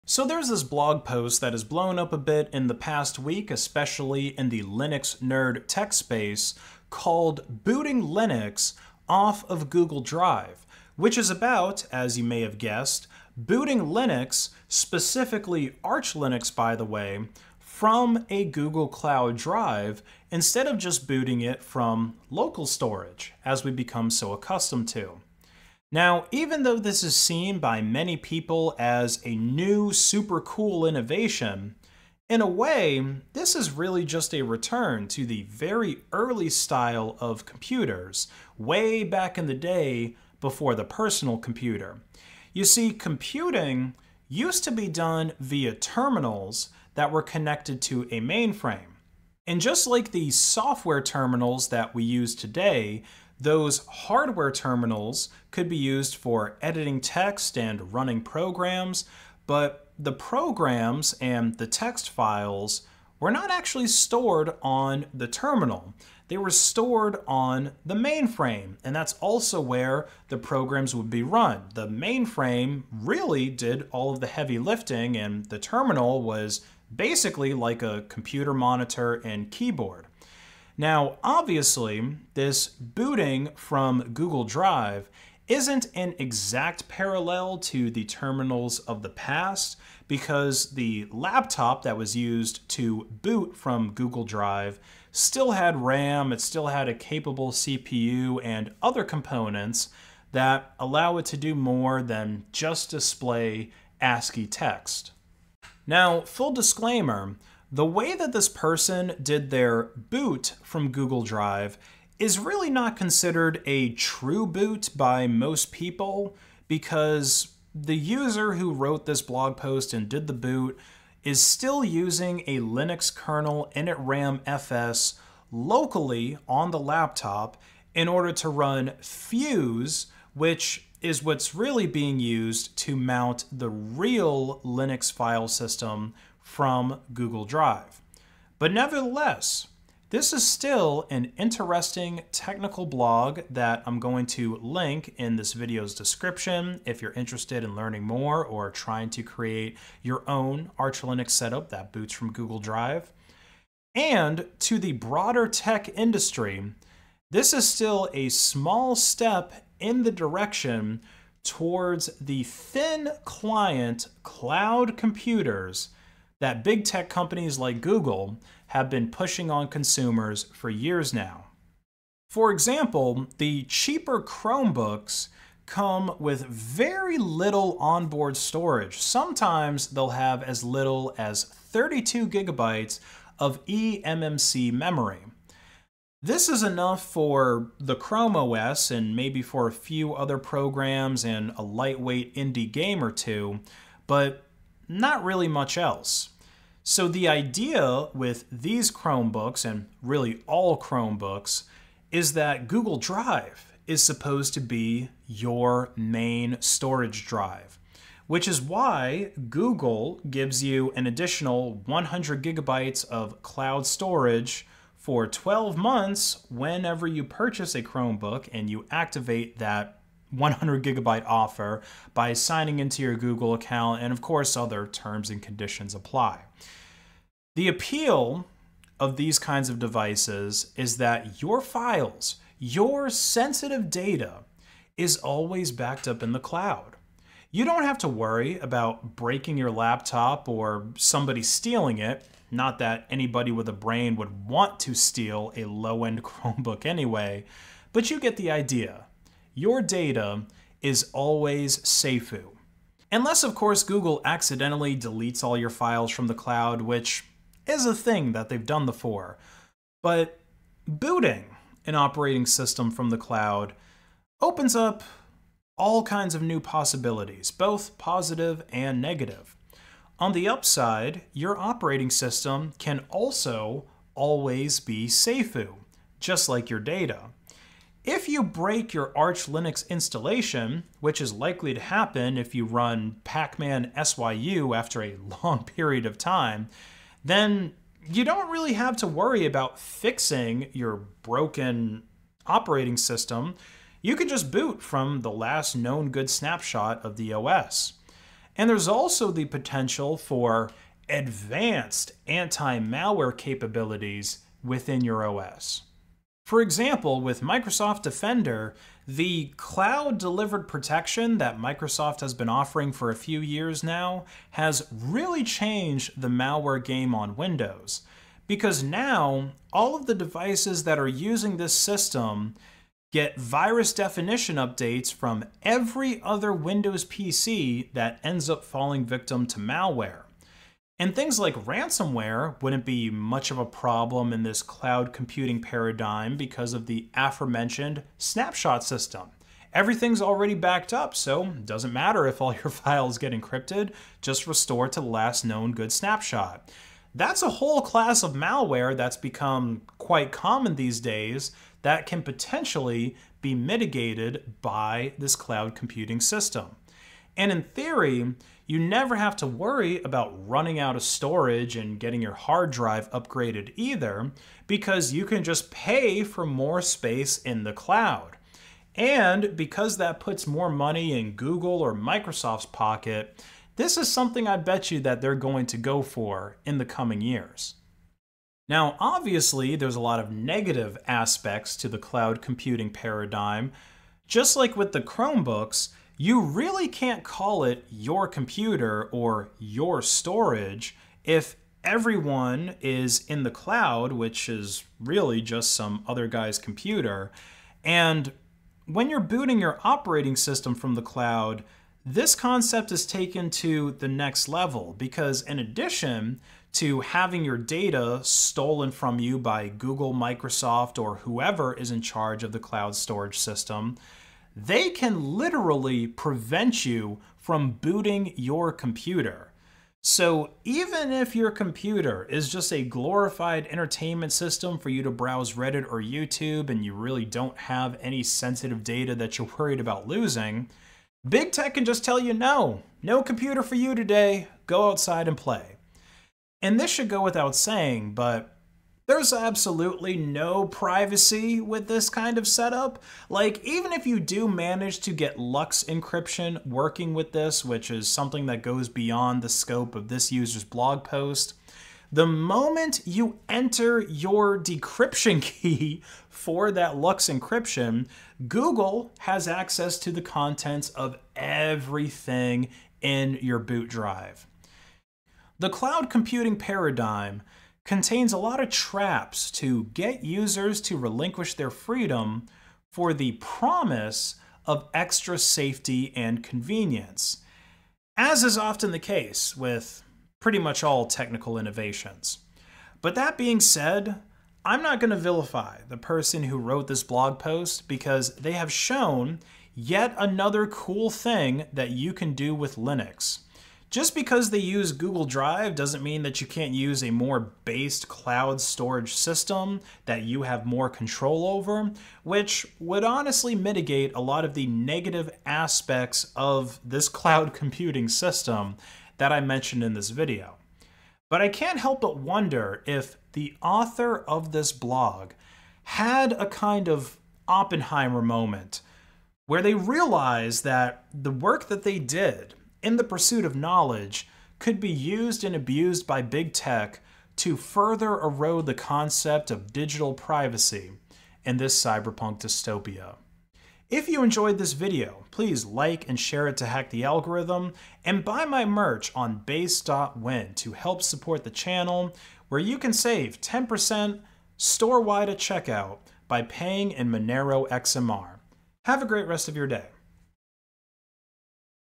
Booting Arch Linux From Google Drive
In this video I discuss a recent blog post about booting Arch Linux from Google drive, and the implications of such a feat for the future of computing.

Monero
45F2bNHVcRzXVBsvZ5giyvKGAgm6LFhMsjUUVPTEtdgJJ5SNyxzSNUmFSBR5qCCWLpjiUjYMkmZoX9b3cChNjvxR7kvh436

Bitcoin
3MMKHXPQrGHEsmdHaAGD59FWhKFGeUsAxV

Ethereum
0xeA4DA3F9BAb091Eb86921CA6E41712438f4E5079

Litecoin
MBfrxLJMuw26hbVi2MjCVDFkkExz8rYvUF